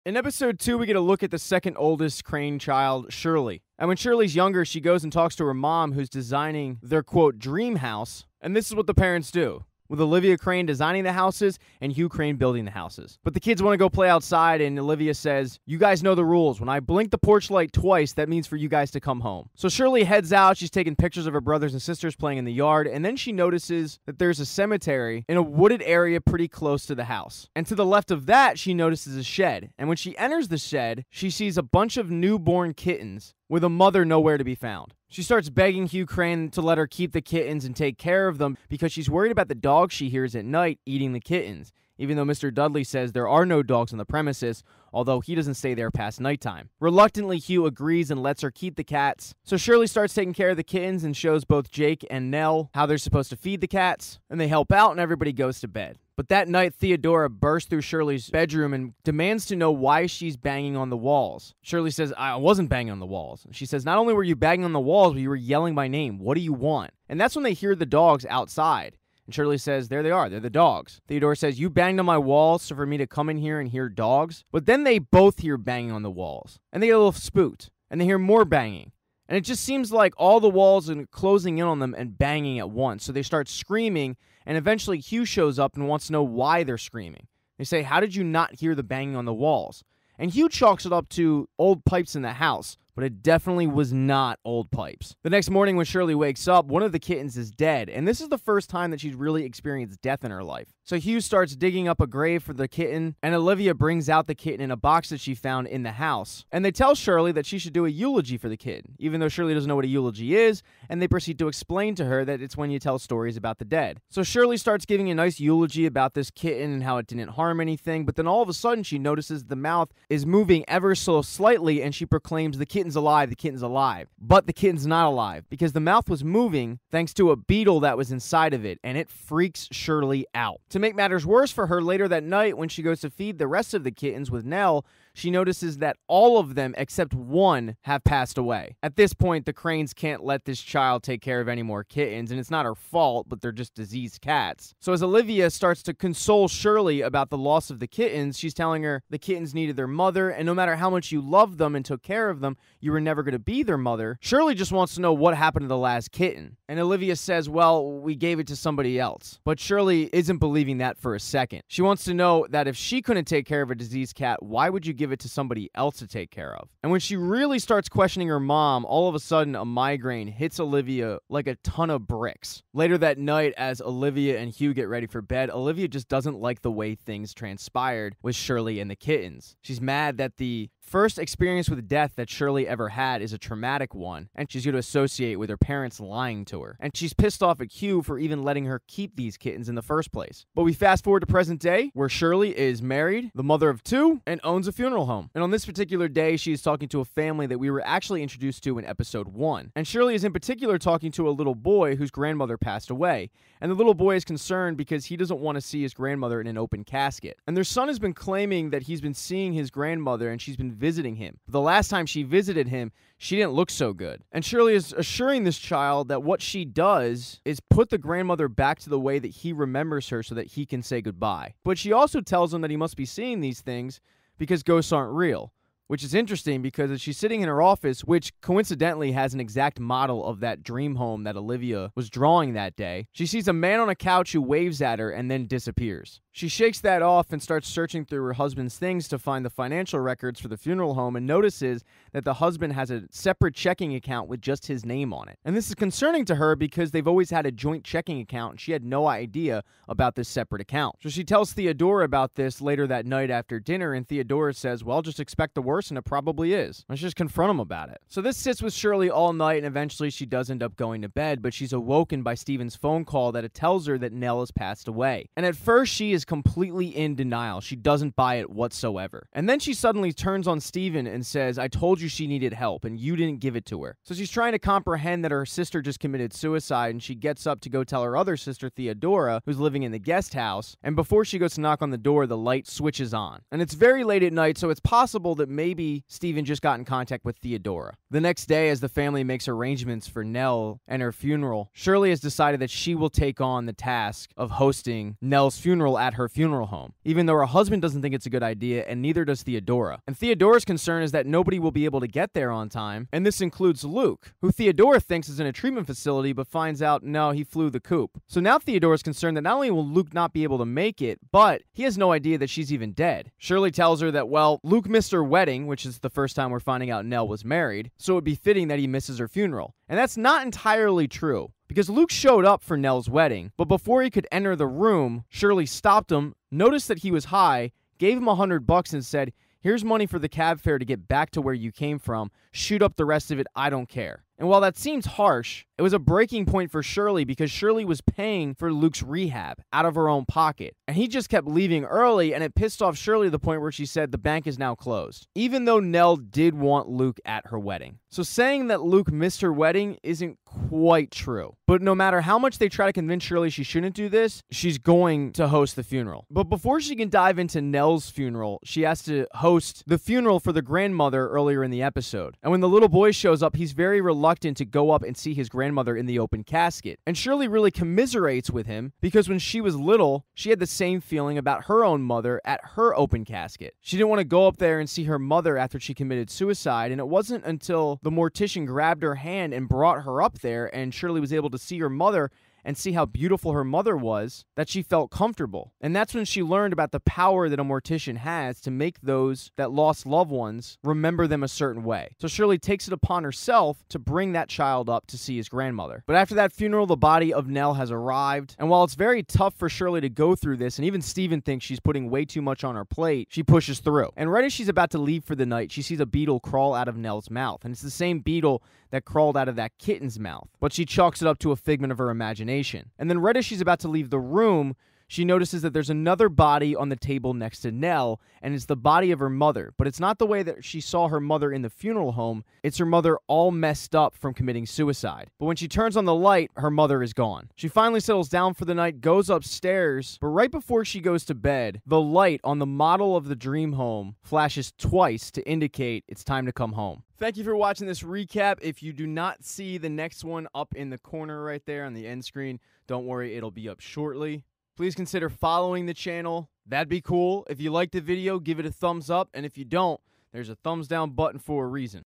The Haunting Of Hill House Episode 2 Open Casket Recap
A recap of the second episode of The Haunting of Hill House Open Casket